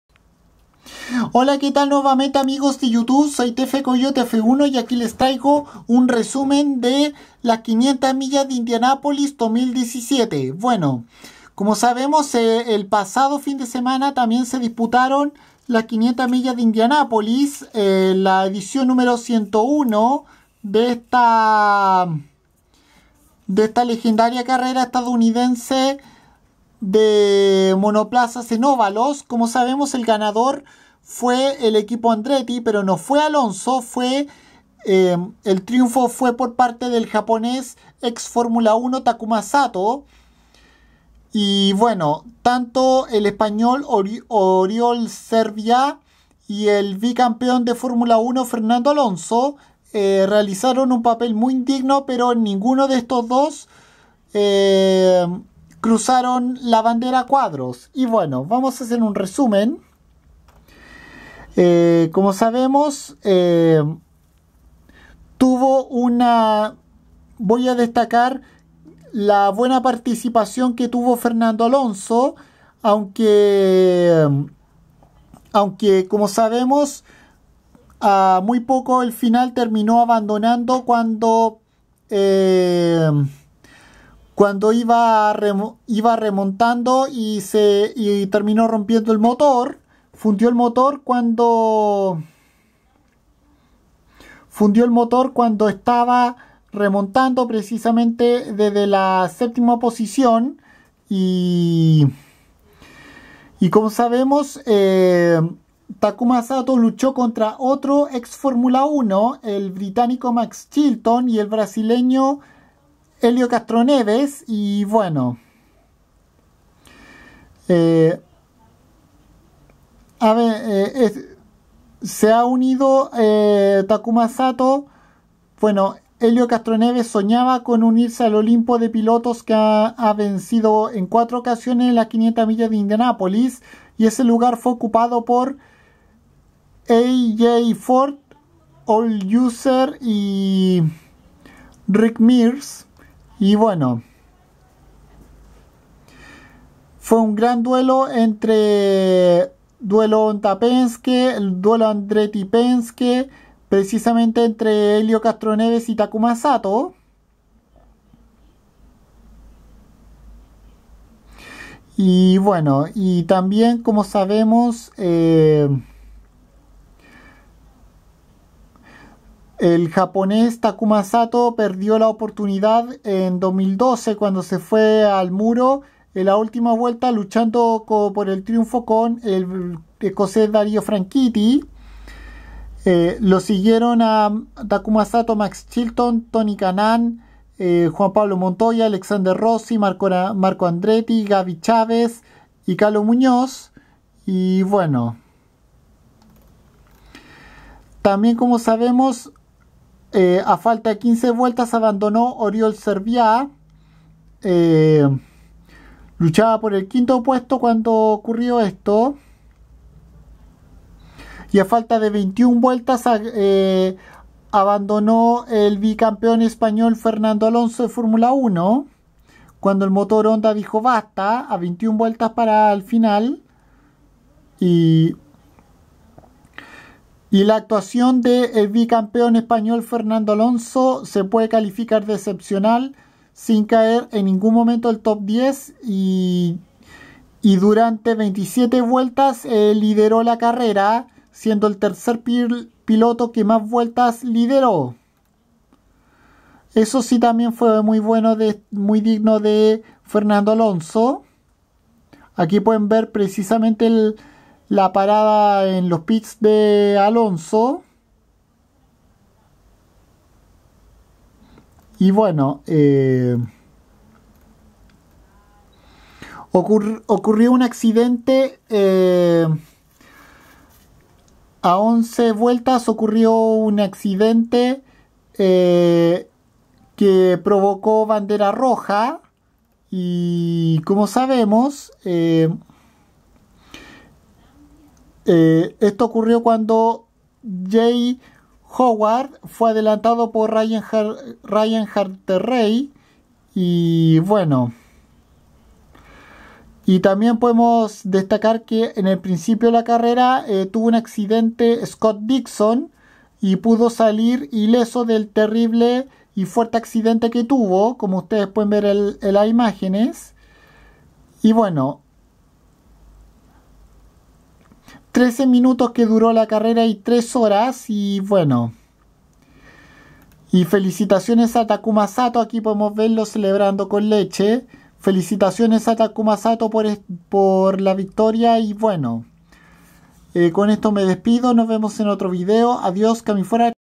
Resumen de las 500 Millas de Indianapolis 2017
En este vídeo a través de fotos e información les hago un resumen de las Indy 500 en su edición n° 101 donde salió victorioso el japonés Takuma Sato en Andretti y el bi campeón español de F1 Fernando Alonso tuvo una increíble actuación a pesar de que reventó motor a solo 21 vueltas del final mientras ocupaba en 7° lugar